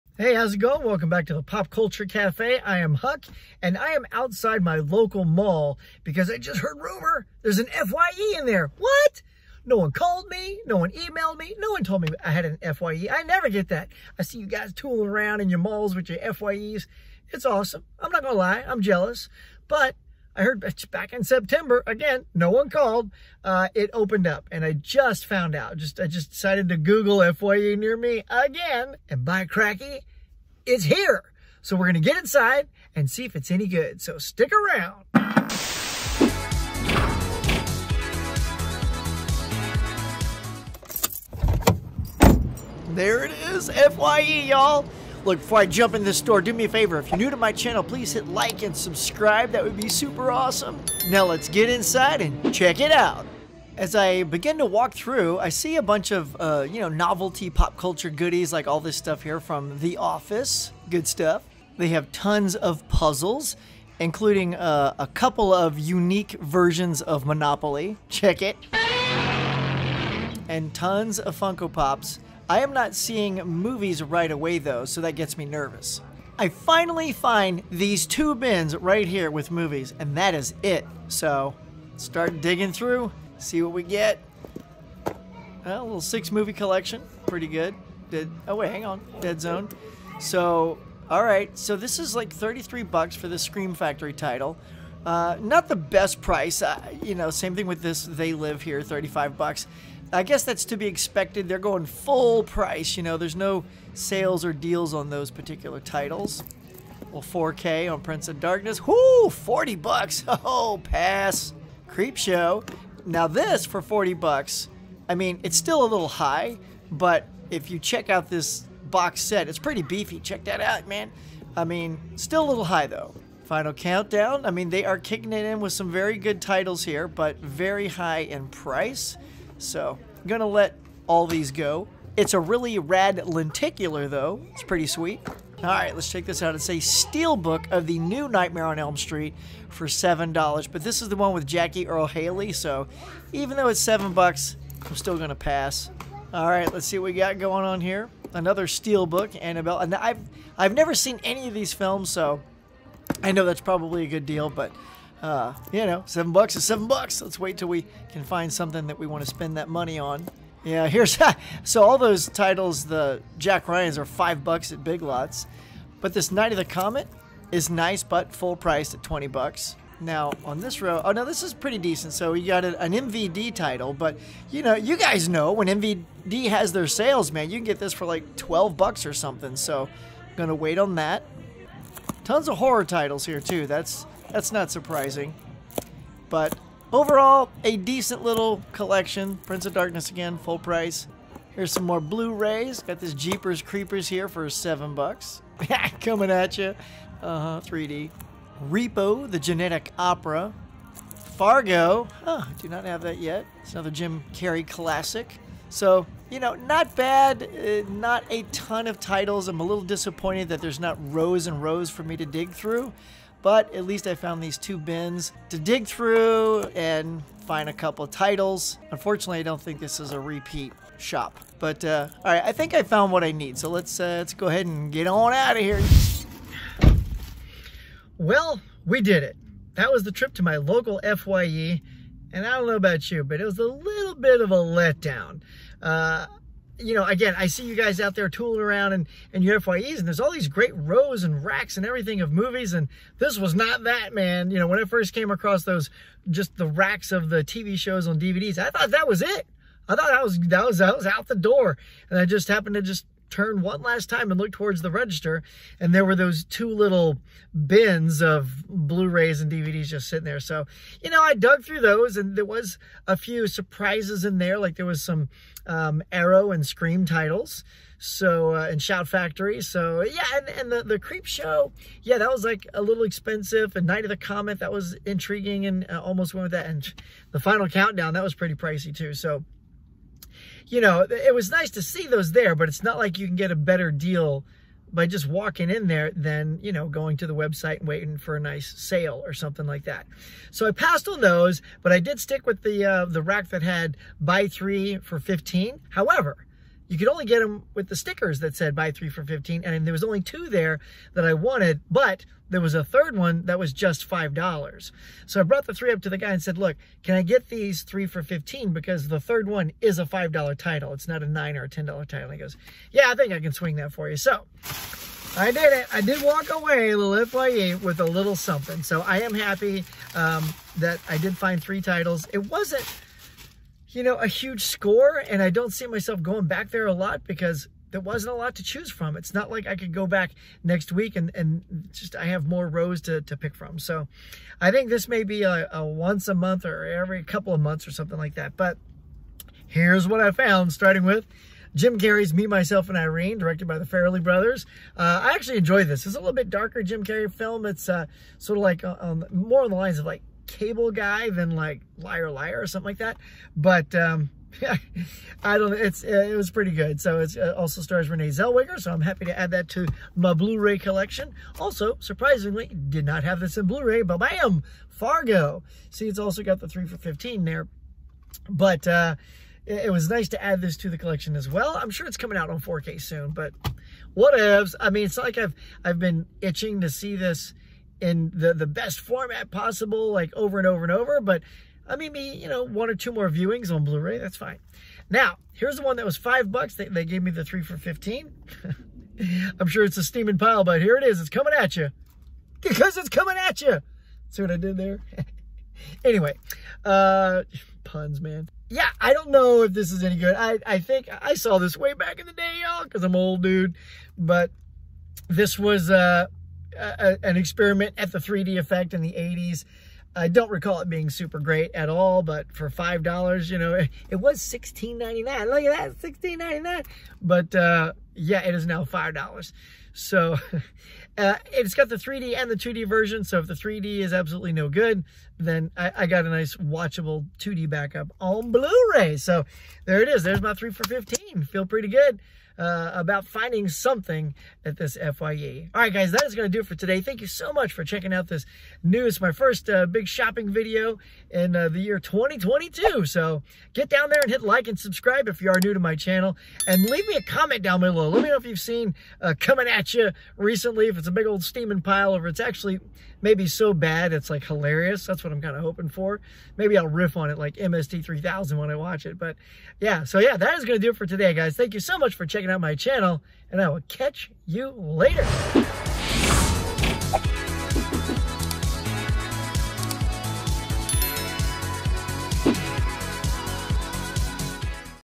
My First FYE Run in 2022!!
So for the first time, an FYE has opened up in my area. I'm very excited to see if this location lives up to all the hype. So join me for my first blu-ray hunt of 2022 at my local FYE!!